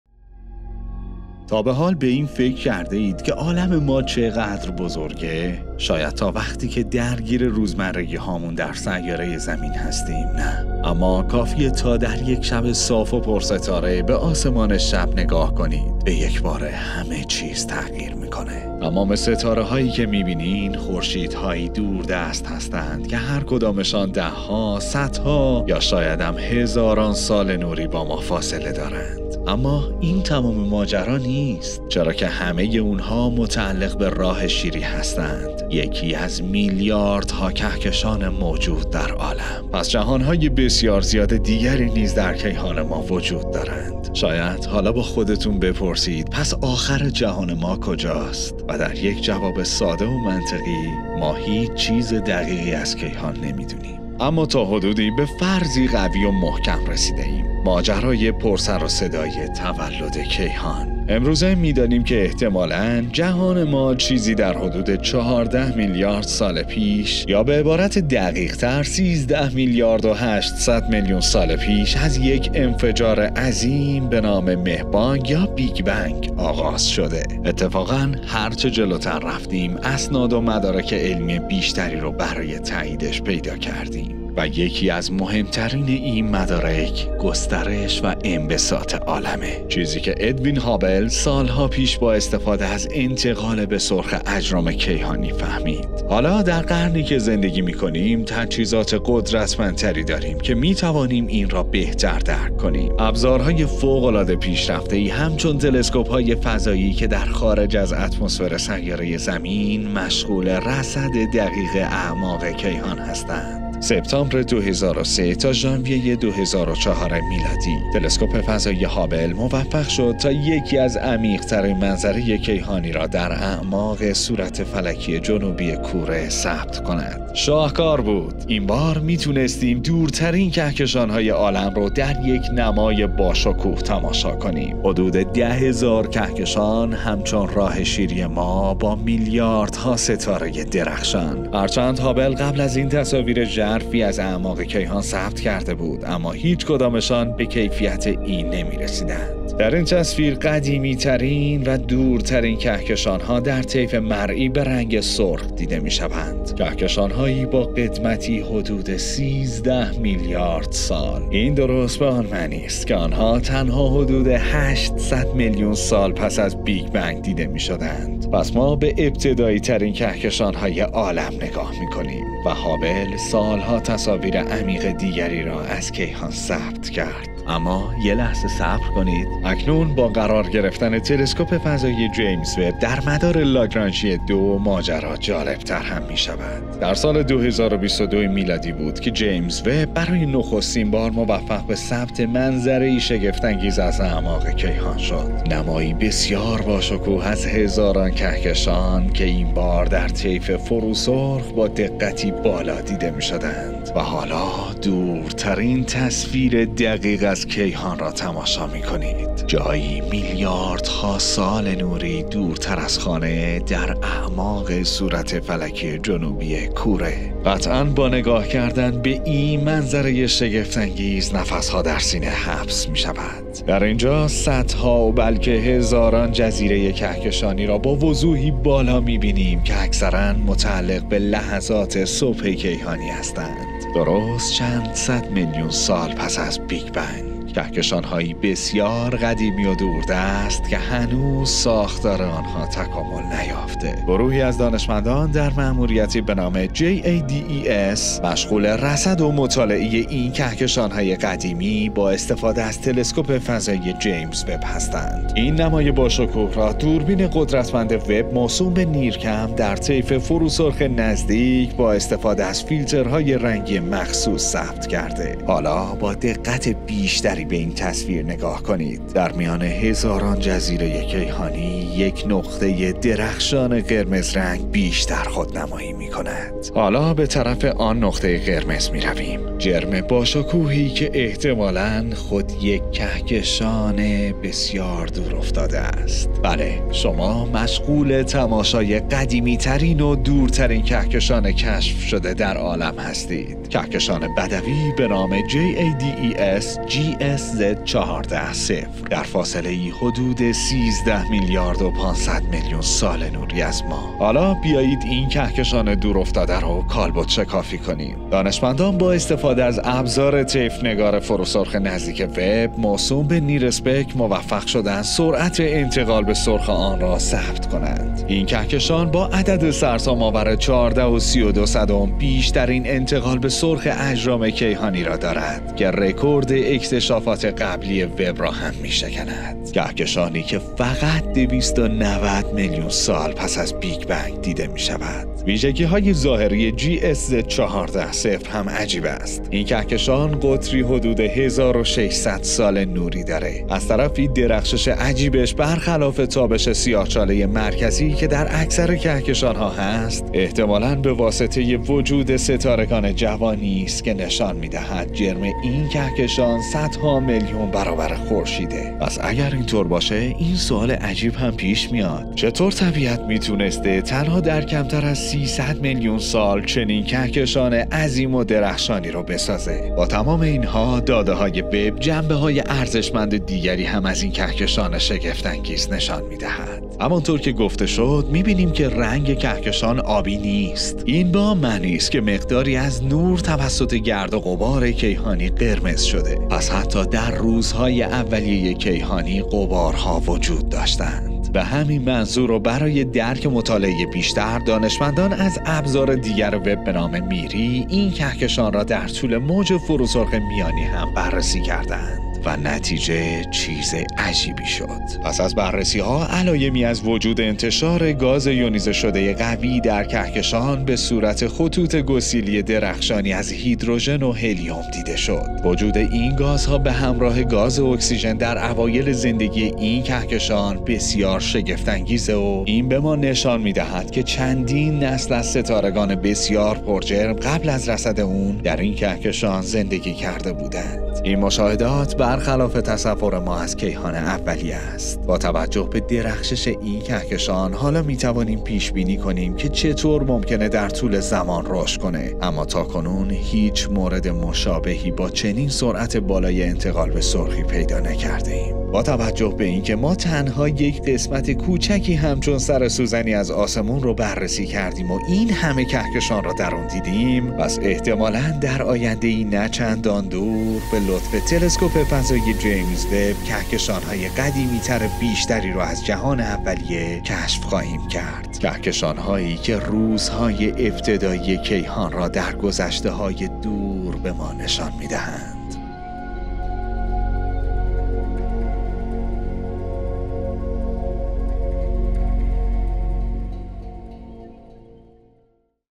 جیمزوب قدیمی ترین کهکشان عالم را کشف کرد
به تازگی تلسکوپ فضایی جیمزوب موفق شد تا با نگاهی دقیق در اعماق صورت فلکی جنوبی کوره دورترین و قدیمی ترین کهکشان عالم را به نام JADES-GS-z14-0 را کشف کند. این کهکشان در فاصله ای حدود 13.5 میلیارد سال نوری از سیاره زمین قرار دارد و تنها حدود 300 میلیون سال پس از انفجار بزرگ بیگ بنگ دیده می شود. ایهن کهکشان با سرعت باورنکردنی عدد 14.32 در حال انتقال به سرخ است، چیزی که گسترش و انبساط جهان را تایید می کند.